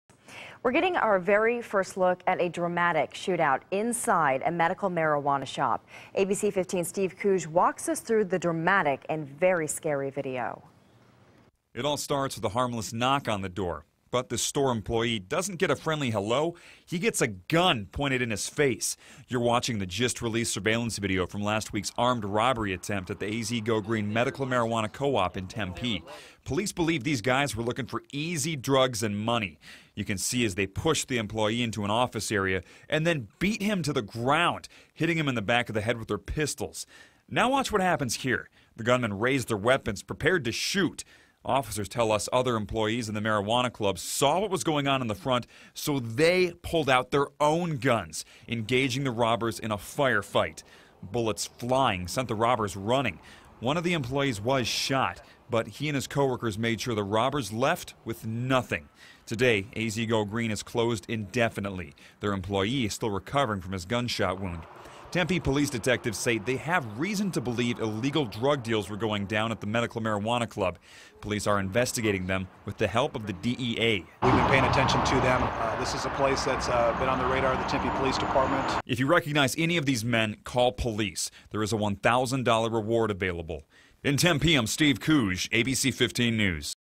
video shows shooting at Tempe medical marijuana co-op
Newly released surveillance video captured a shooting that left one person injured at a Tempe medical marijuana co-op.